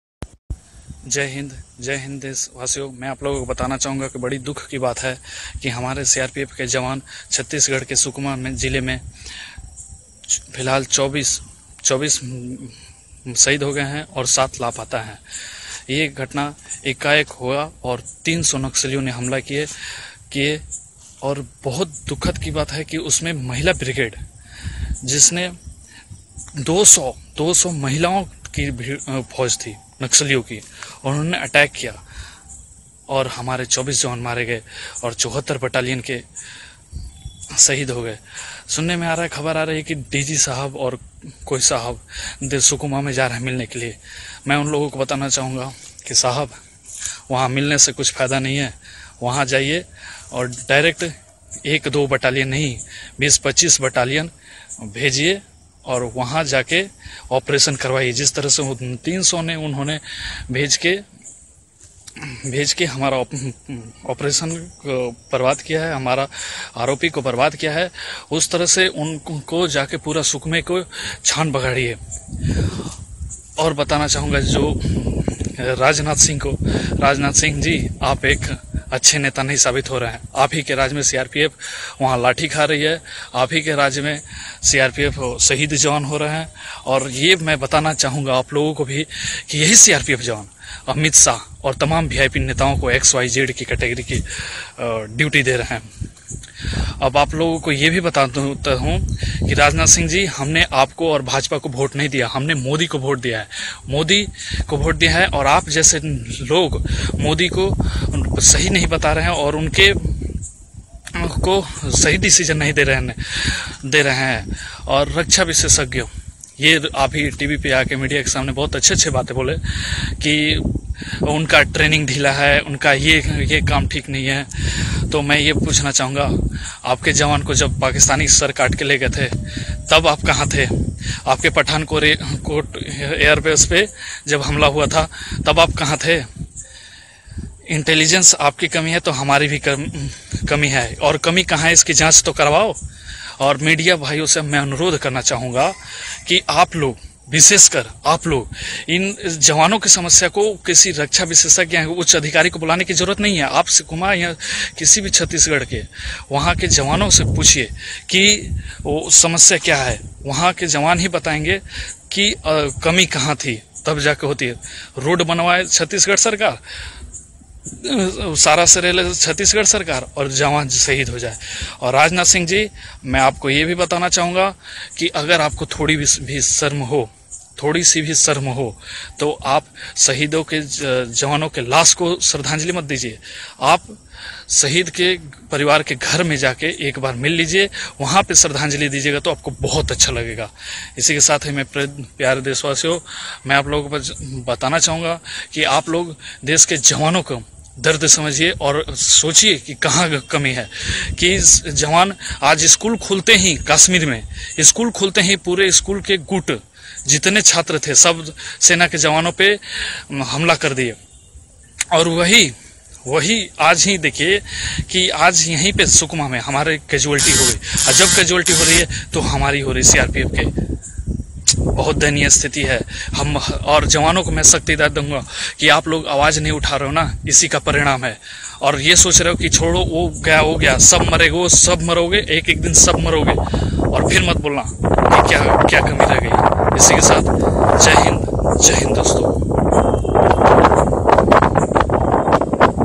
CRPF Jawaan Pankaj Mishra questions central govt after Sukma ambush
CRPF Jawan on his Facebook live, blames Home Minister Rajnath Singh for the failure in Sukma Naxal Attack and deteriorating condition of Armed Forces.